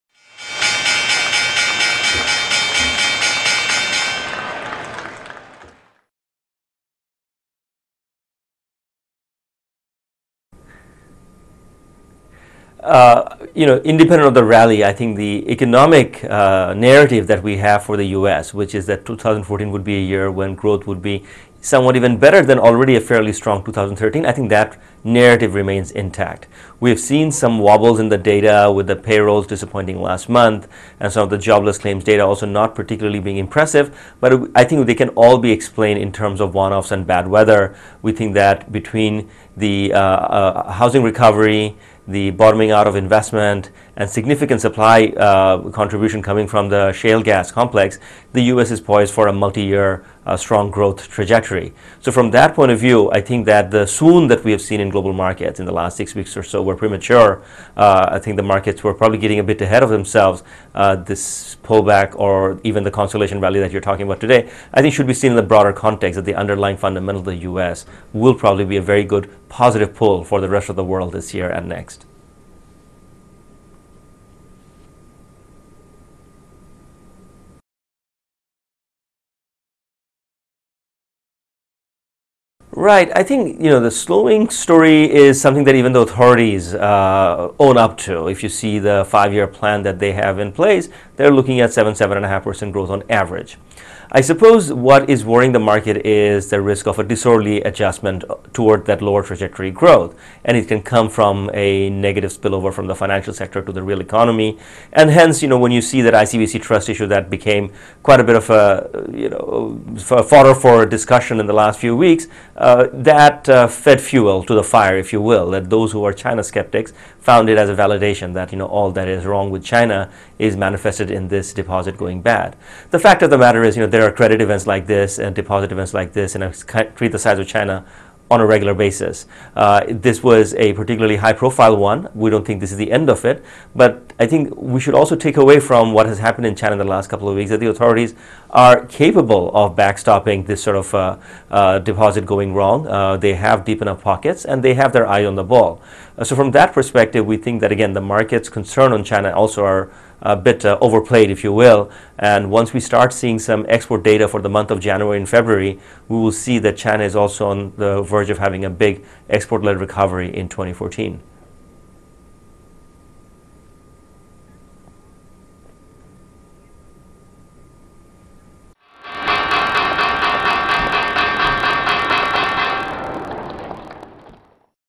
Markets' concern on China is overplayed and investors should see a big export-led recov...
MARKETS' CONCERN ON CHINA IS OVERPLAYED AND INVESTORS SHOULD SEE A BIG EXPORT-LED RECOVERY IN THE COUNTRY'S ECONOMY IN 2014, SAYS TAIMUR BAIG OF DEUTSCHE BANK AG

SHOWS: SINGAPORE (FEBRUARY 7, 2014) (REUTERS - ACCESS ALL)
TAIMUR BAIG, CHIEF ECONOMIST ASIA, DEUTSCHE BANK AG
1. (QUESTION GRAPHIC)
"How sustainable is the rally in the U.S. stock market?"
2.  TAIMUR BAIG SAYING:
"You know, independent of the rally, I think the economic narrative that we have for the U.S. - which is that 2014 would be a year when growth would be somewhat even better than already a fairly strong 2013 - I think that narrative remains intact. We have seen some wobbles in the data with the payrolls disappointing last month. And so the jobless claims data also not particularly being impressive, but I think they can all be explained in terms of one-offs and bad weather. We think that between the housing recovery, the bottoming out of investment and significant supply of contribution coming from the shale gas complex, the U.S. is poised for a multi-year strong growth trajectory. So from that point of view, I think that the swoon that we have seen in global markets in the last six weeks or so were premature. I think the markets were probably getting a bit ahead of themselves. This pullback or even the consolation value that you were talking about today, I think it should be seen in a broader context of the underlining fundamental of the U.S. will probably be a very good positive pull for the rest of world this year and next."
3. (QUESTION GRAPHIC)
"Why are investors still worried about a China slowdown?"
4.  TAIMUR BAIG SAYING:
"Right. I think you know the slowing story is something that even the authorities own up to. If you see the five-year plan that they have in place; they are looking at seven-seven and a half percent growth on average. I suppose what is worrying the market is the risk of a disorderly adjustment toward that lower trajectory growth; anything come from a negative spillover from the financial sector to the real economy and hence when you see that the ICBC trust issue that became quite a bit of fodder for a discussion in the last few week...